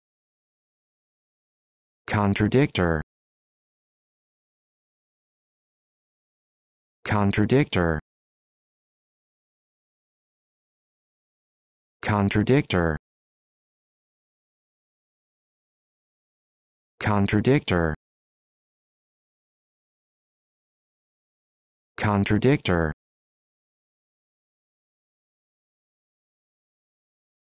How to Pronounce Contradicter
This video shows you how to pronounce Contradicter